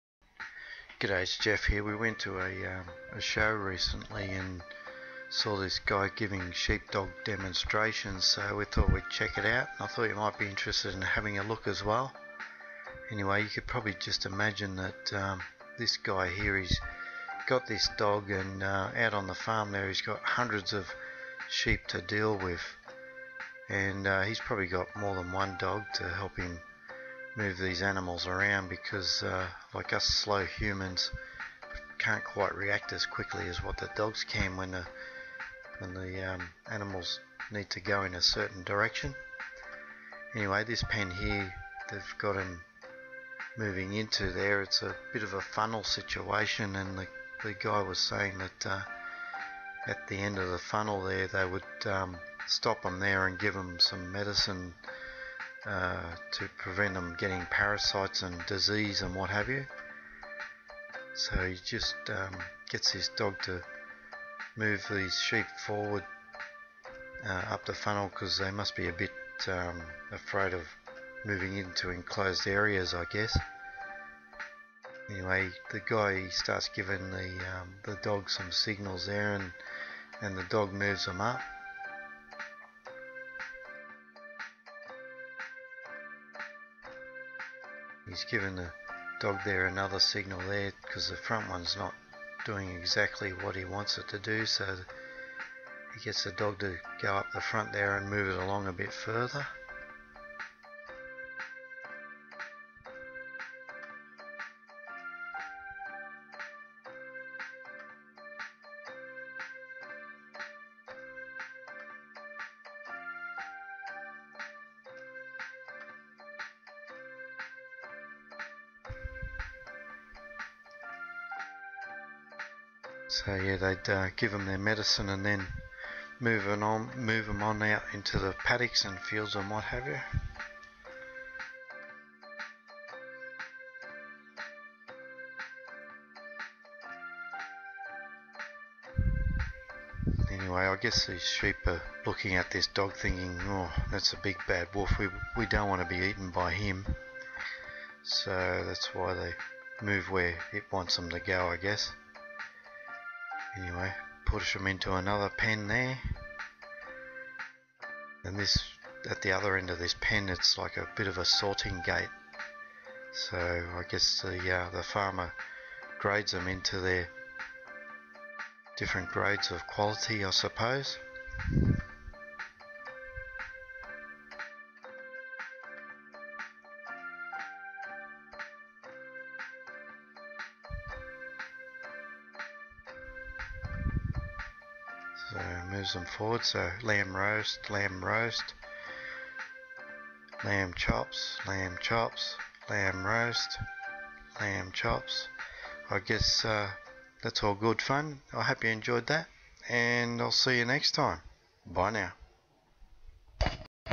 A MAN AND HIS DOG SHEEP DOG DEMONSTRATION
A MAN AND HIS DOG . SHEEP DOG DEMONSTRATION .  THIS IS A SMALL LOOK INTO HOW THESE DOG ARE SO HELPFUL TO THE FARMERS .